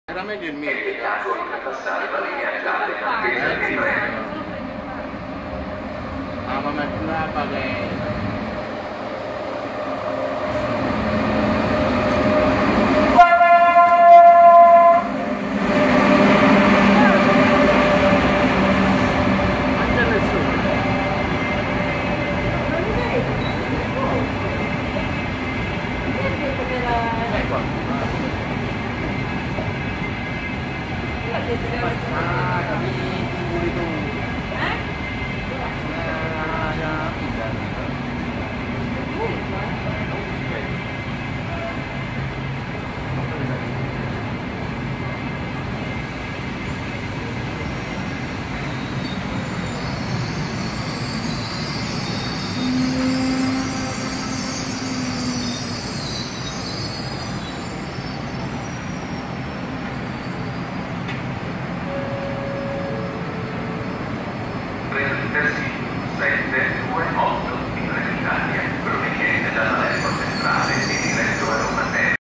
IL PRIMO FRECCIA ROSSA IN ARRIVO ALLA STAZIONE DI SAPRI !
L'11 Giugno 2017 con l'entrata del nuovo orario estivo viene effettuato il primo collegamento con servizio Freccia Rossa da Milano C.le a Sapri esclusivamente nei week-end fino al 17 Settembre . Qui in arrivo nella Stazione di Sapri dove molte persone hanno accolto con felicità ed entusiasmo il nuovo e primo collegamento . Il convoglio in questione è l'ETR 500 nº11 .